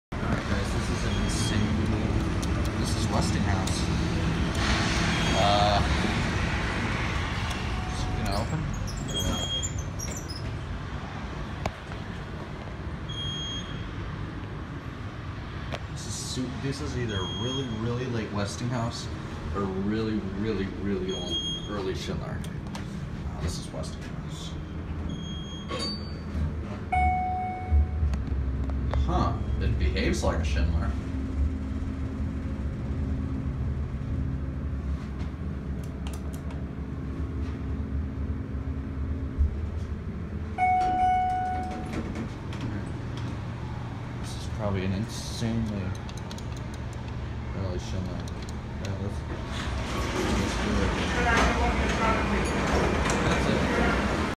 WEIRD Westinghouse Hydraulic Elevator @ Ft Lauderdale-Hollywood Intl Airport
TECH SPECS:
Brand: Westinghouse
Model: N/A
Drive: Hydraulic
Year Installed: 1980s
Floors Served: 2
Capacity: 3500 lbs
Speed: 100 FPM
Doors: Center Opening

FIXTURES:
Buttons: Westinghouse AE
Indicator: Westinghouse AE Analog
Lanterns: Westinghouse RT